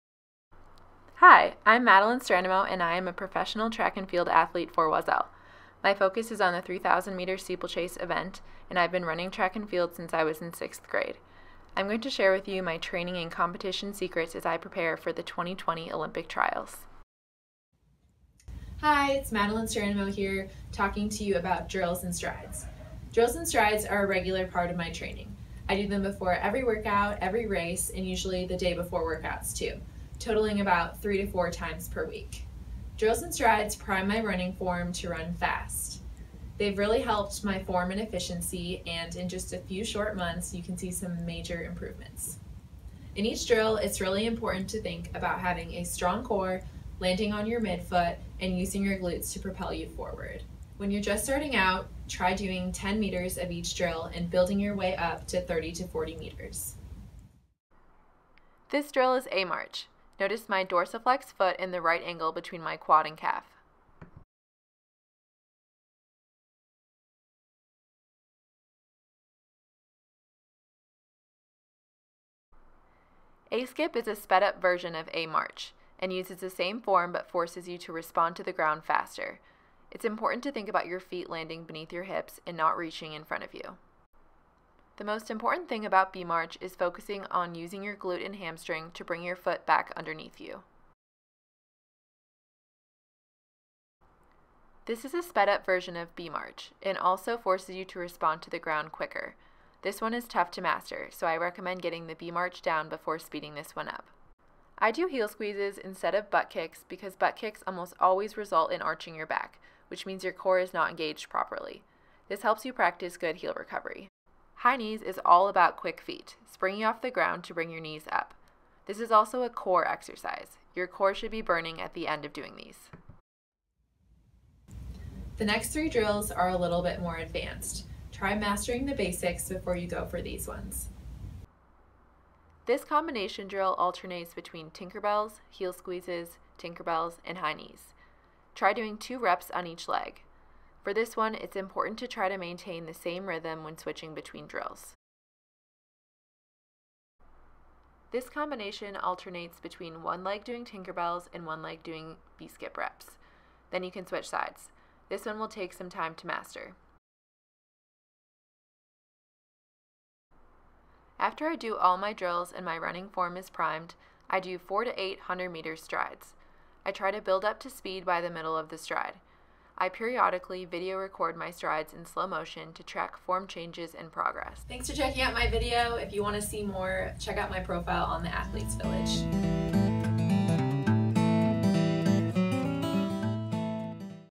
Oiselle professional track & field athlete Madeline Strandemo discusses her favorite running drills.
In this instruction video, Oiselle professional track & field athlete Madeline Strandemo shows us the different running drills that she does to perfect her running form.  Madeline also talks about how often she does these running drills and what benefits she gets from her running drills.